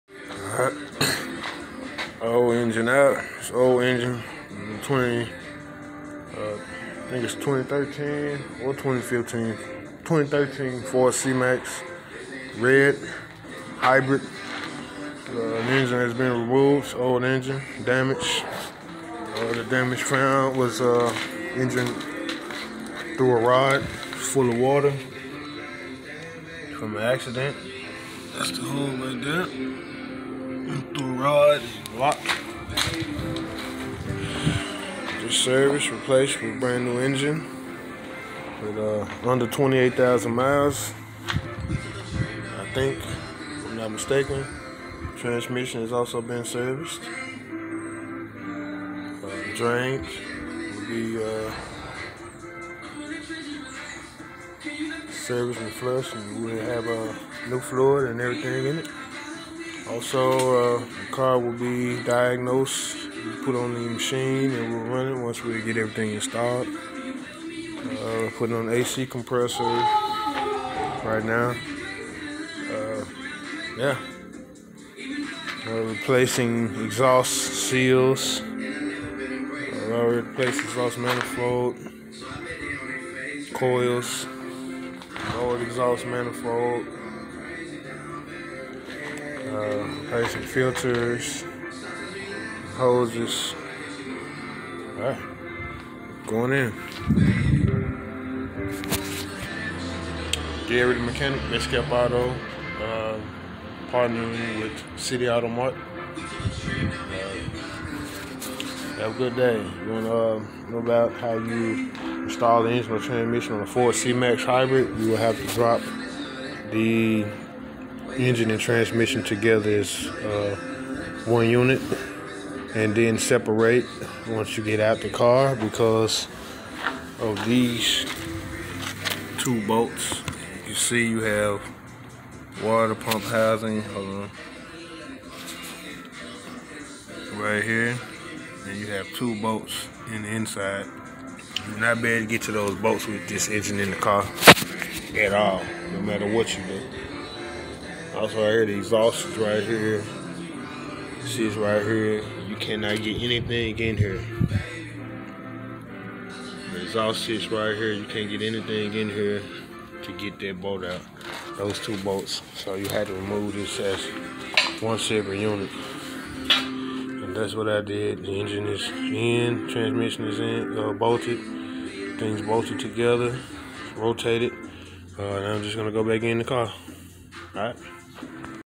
Ford C-Max Hybrid engine install how to advice 2013 and up
here in this video tutorials showcase the removal and install of the engine and oil transmission in a Ford C-Max. to install the engine or transmission you will have to remove both as one unit.
there's no other way you can re separate the engine and transmission from this car.
here you will unattached all electrical connections including the inverter first remove the splash Shields you also remove the splash cover for the windshield and windshield wipers to gain access to the rear inverter bolts to remove the power inverter.
then you will disconnect all steering suspension components including.
you will separate axles from both sides of transmission and engine mount on the rear of the engine.